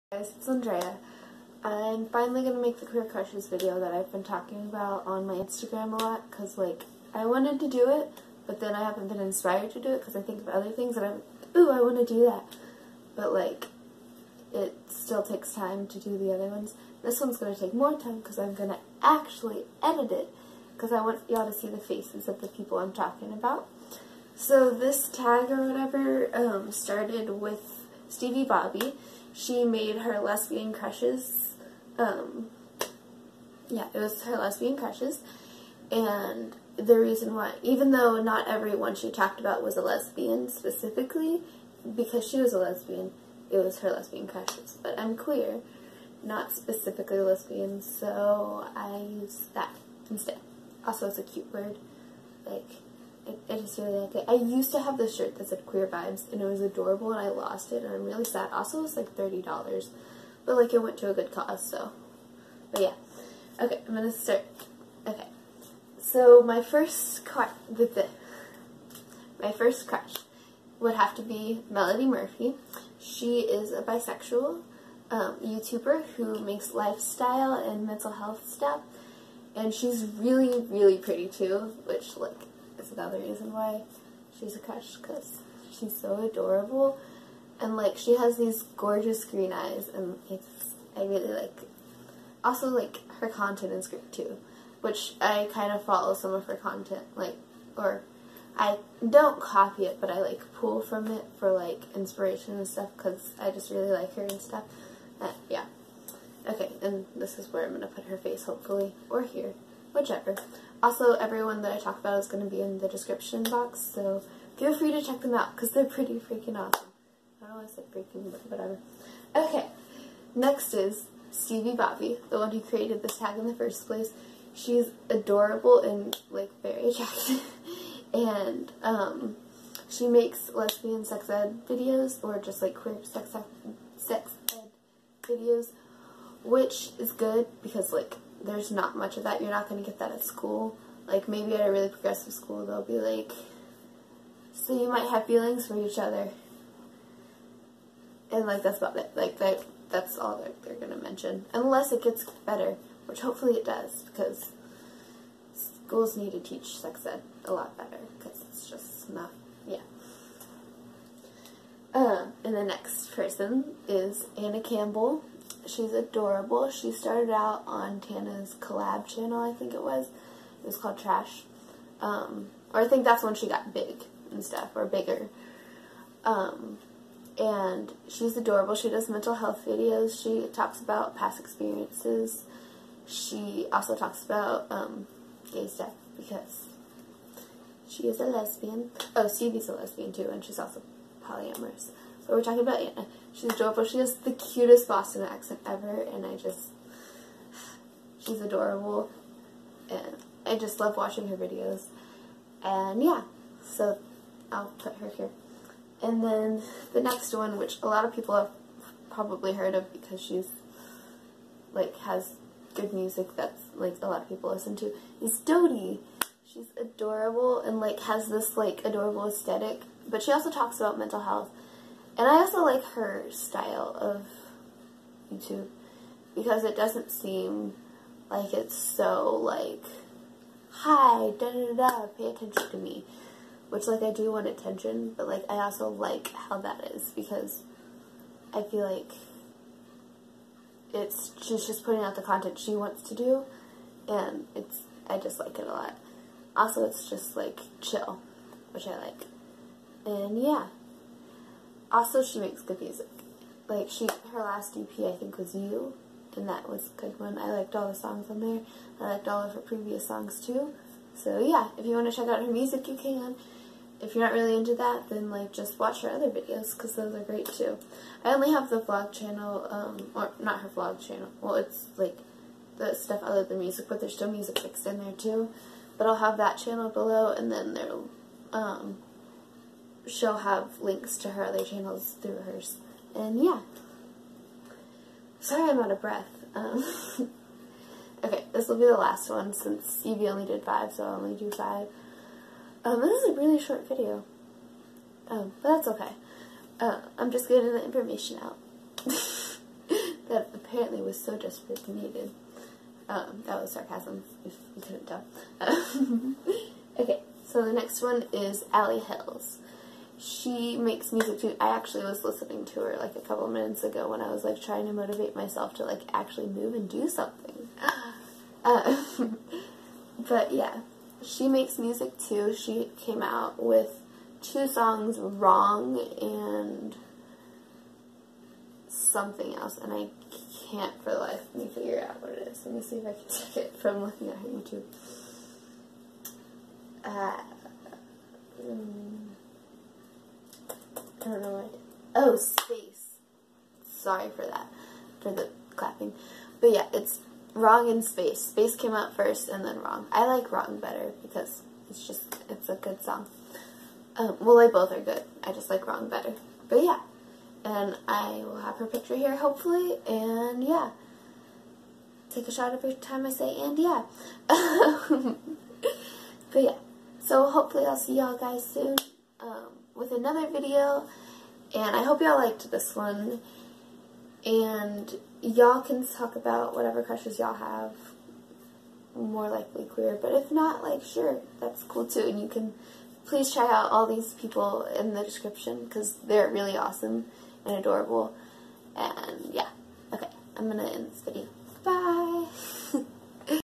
My Queer Crushes tag
I did Stevie Bobbie's My Lesbian Crushes tag!
Here are all the lovely people I mentioned in this video.